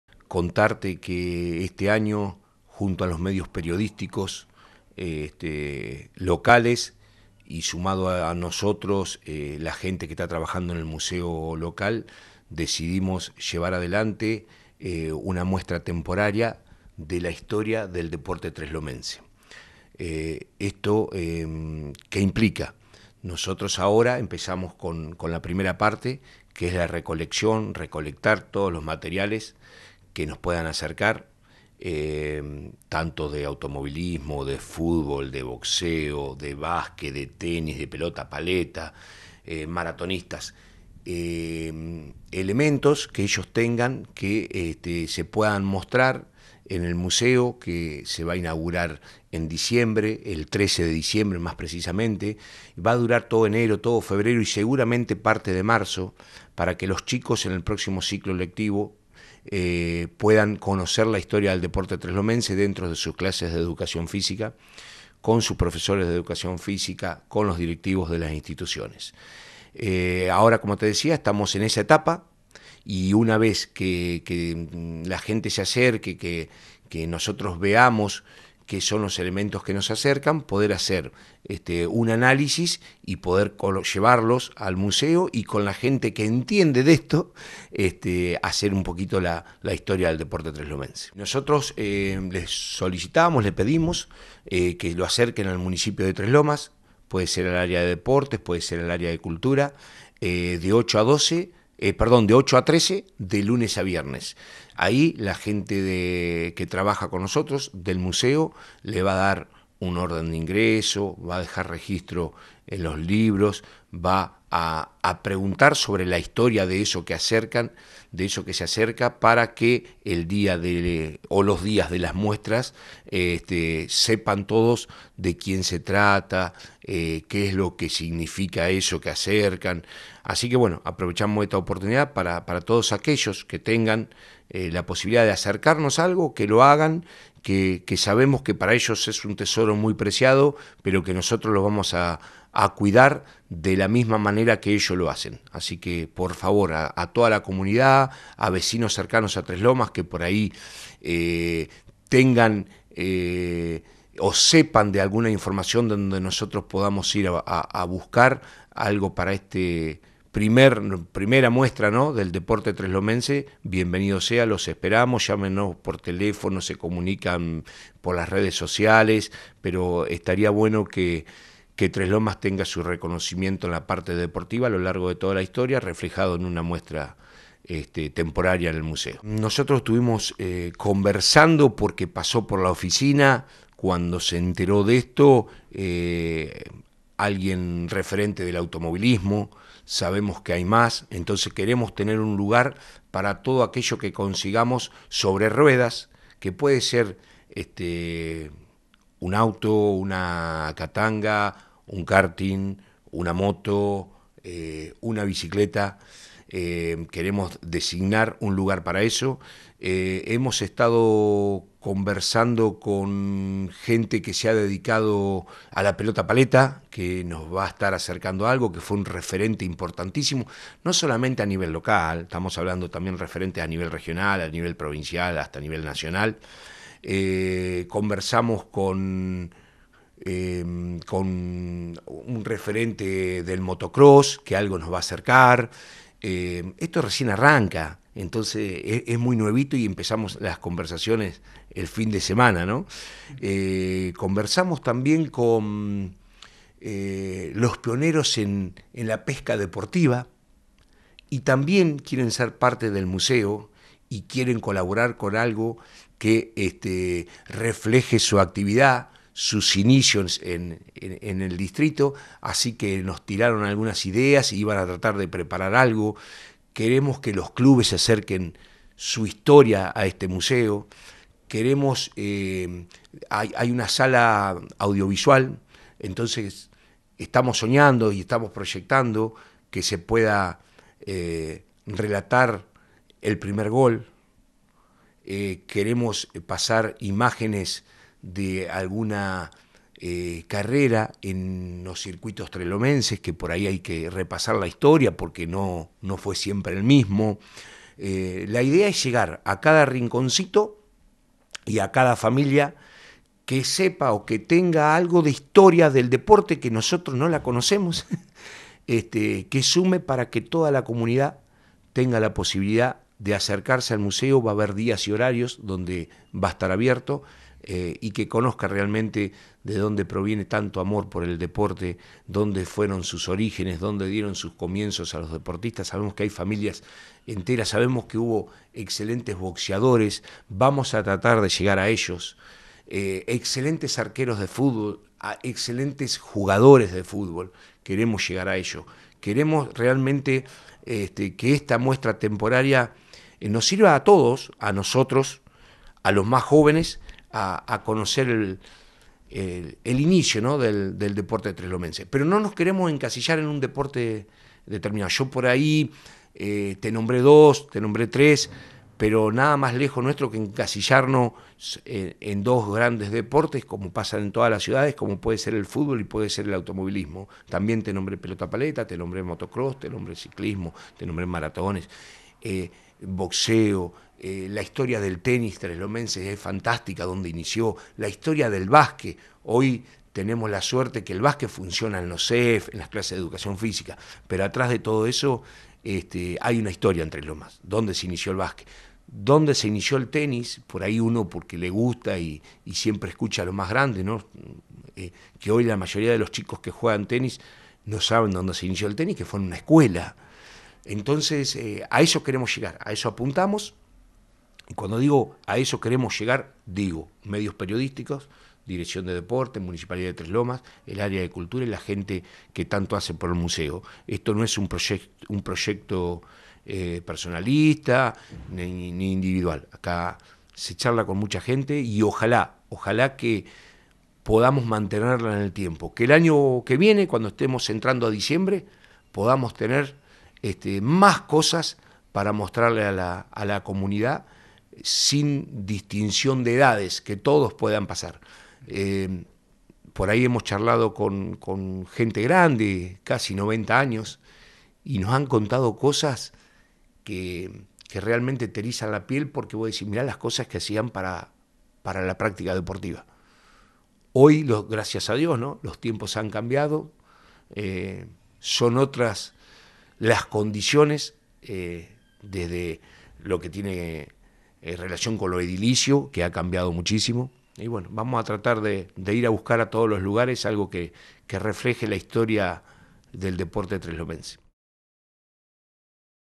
Se viene la primera muestra del deporte treslomense
Exhibirá piezas que reflejen lo mejor de cada momento y personajes locales.